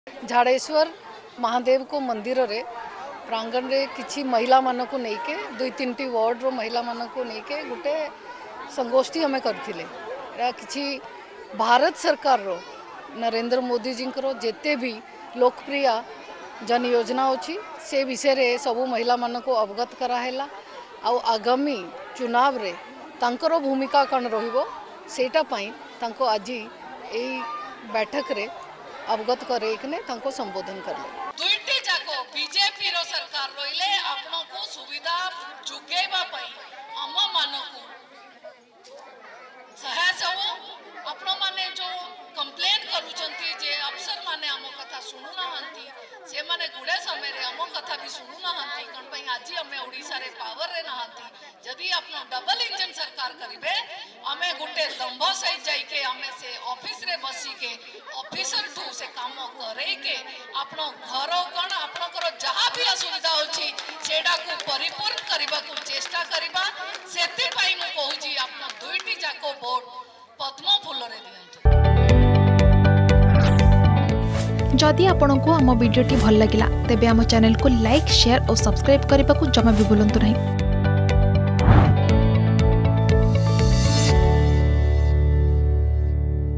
ନାରୀ ଶକ୍ତି ବନ୍ଦନ କାର୍ଯ୍ୟକ୍ରମରେ ଯୋଗ ଦେଲେ ମୃଦୁଳା ଠାକୁର ପ୍ରଧାନ || Deogarh
ଦେବଗଡରେ ଅନୁଷ୍ଠିତ ନାରୀ ଶକ୍ତି ବନ୍ଦନ କାର୍ଯ୍ୟକ୍ରମରେ ବିକାଶ ଫାଉଣ୍ଡେସନ ଅଧ୍ୟକ୍ଷା ମୃଦୁଳା ଠାକୁରଙ୍କ ଯୋଗଦାନ । ଝାଡେଶ୍ୱର ମନ୍ଦିର ପ୍ରାଙ୍ଗଣରେ ଶତାଧିକ ମହିଳାଙ୍କୁ ନେଇ ନାରୀ ଶକ୍ତି ବନ୍ଦନ କାର୍ଯ୍ୟକ୍ରମ ଆୟୋଜିତ ହୋଇଥିଲା ।